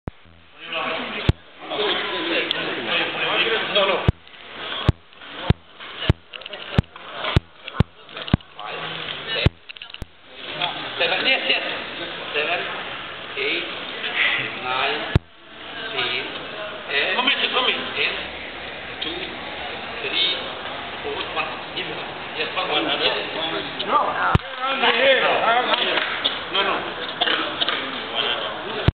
Stockholm con man takes money from victims pocket
In this video, the con man has seen literally taking the money from his mark's wallet.